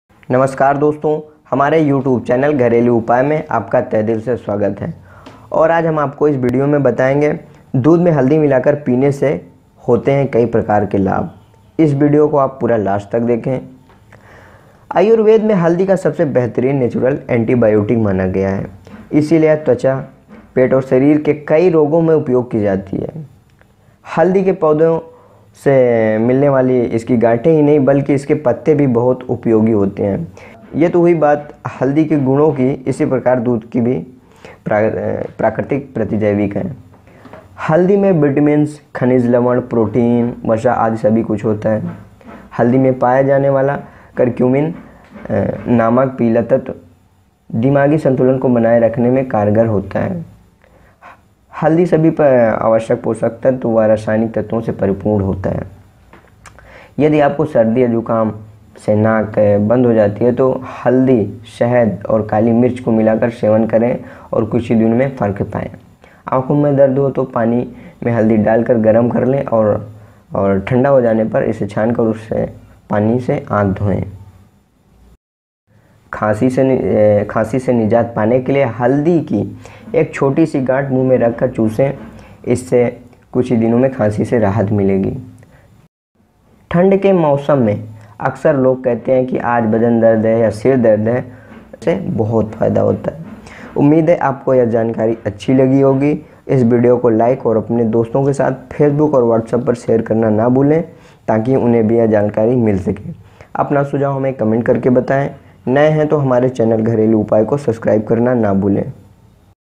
दूध में हल्दी मिलाकर पीने से होते हैं कई लाभ | Health benefits milk with turmeric
आयुर्वेद में हल्दी को सबसे बेहतरीन नैचुरल एंटीबायोटिक माना गया है। इसलिए यह त्वचा पेट और शरीर के कई रोगों में उपयोग की जाती है। हल्दी के पौधे से मिने वाली इसकी गांठें ही नहीं, बल्कि इसके पत्ते भी बहुत उपायोगी होते हैं। ये तो हुई बात हल्दी के गुणों की, इसी प्रकार दूध भी प्राकृतिक प्रतिजैविक है।